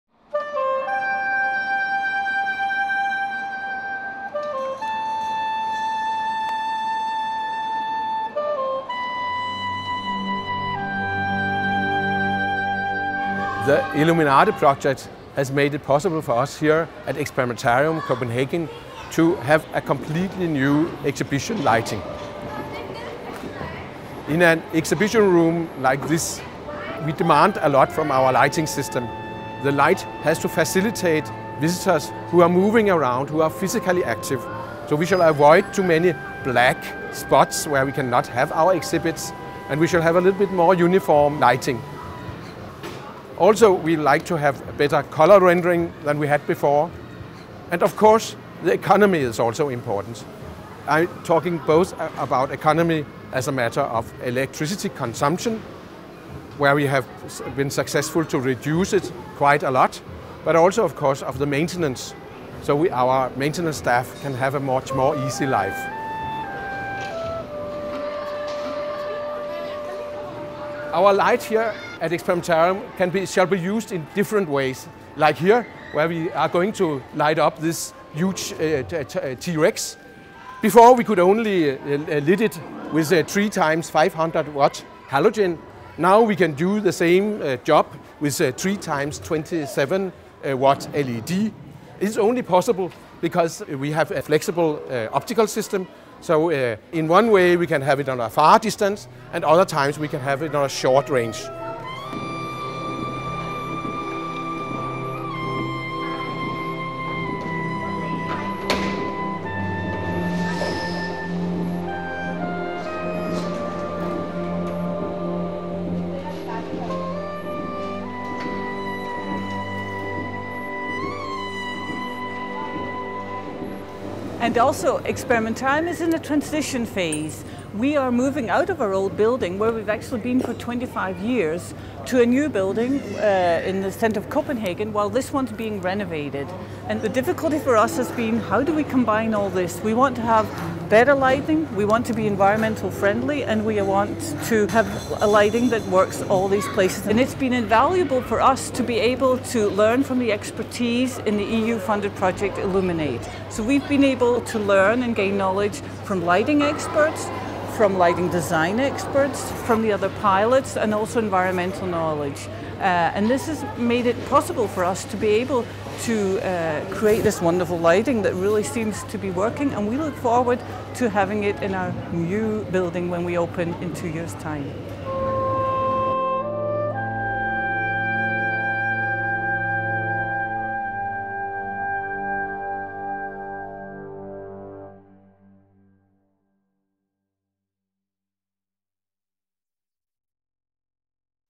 New LED light at Experimentarium
From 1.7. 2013 new exhibition lighting has been installed at the Danish Science Centre Experimentarium. It was made possible due to the participation in the ILLUMINATE project. The light is provided by ERCO and is serving in the "Old" Experimentarium to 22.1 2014. Then it is installed at the temporary Experimentarium City downtown Copenhagen for two years, before it is installed in the new renovated and extended building back home at Tuborg Harbour by the end of 2015.